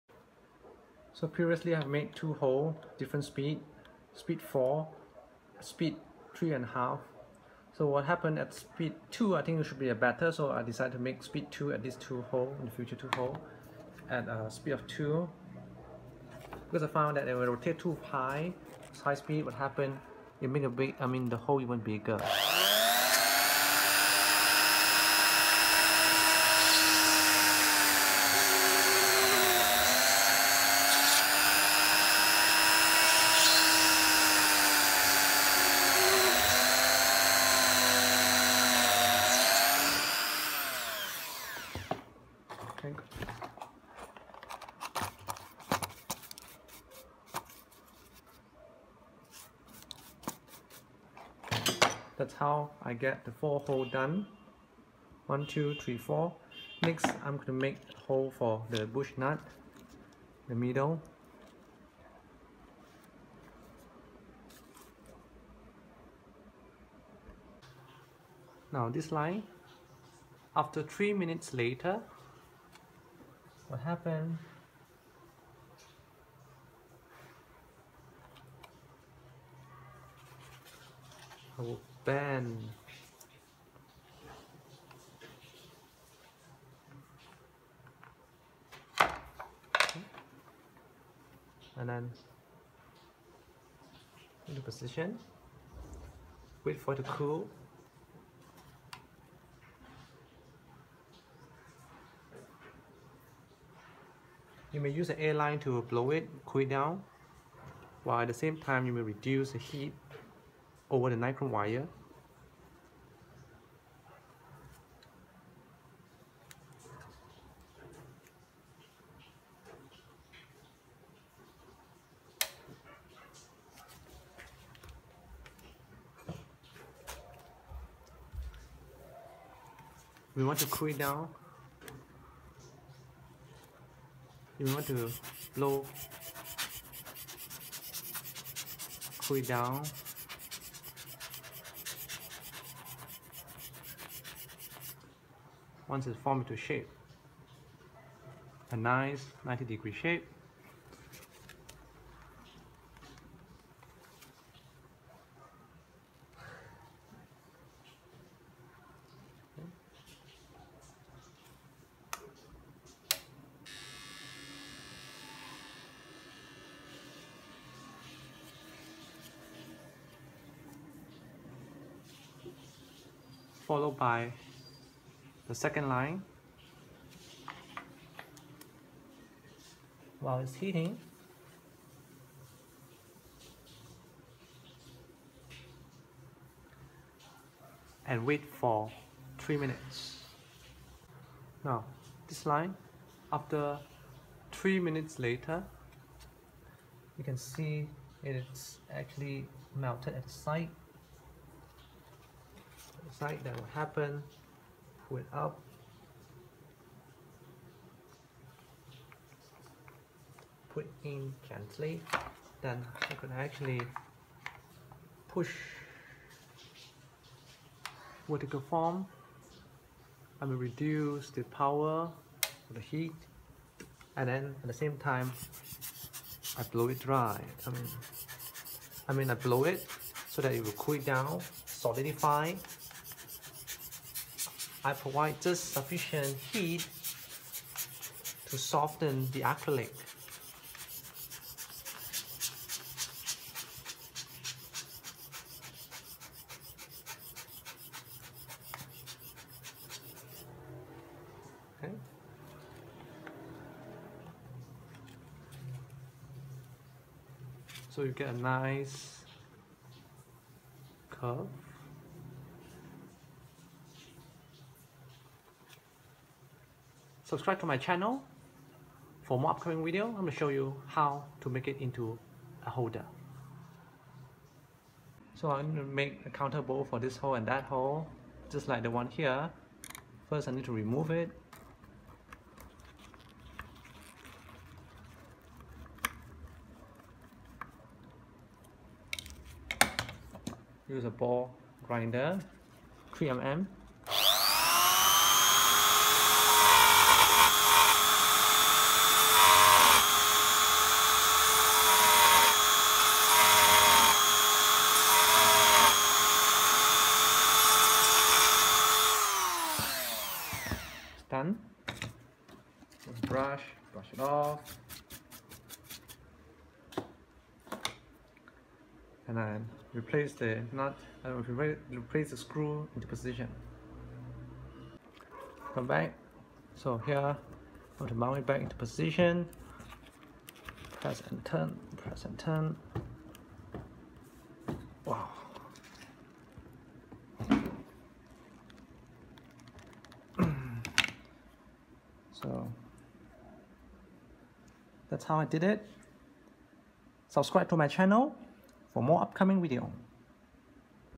Mounting Screws for Acrylic base
This video show how to install 2mm screw and making of counterbore hole.